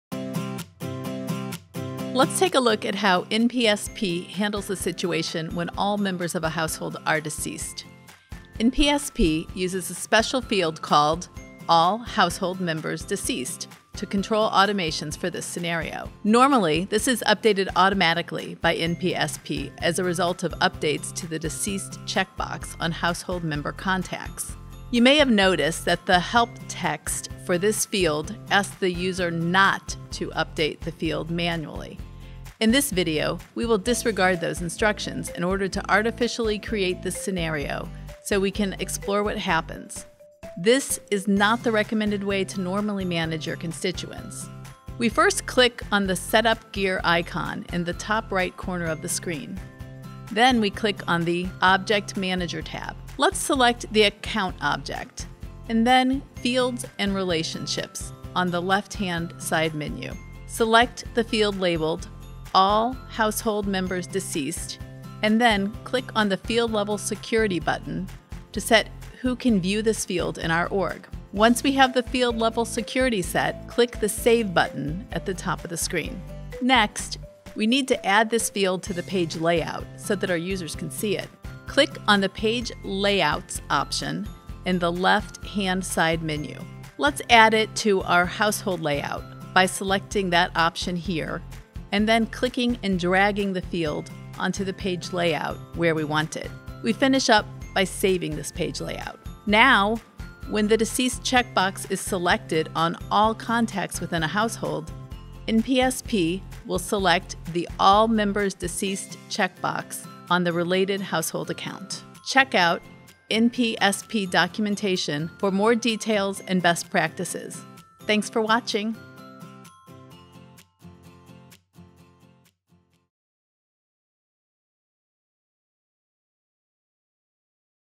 Nonprofit Salesforce How-To-Series: NPSP All Household Members Deceased
This video is walks through how salesforce automatically flags all household members deceased the Nonprofit Success Pack (NPSP).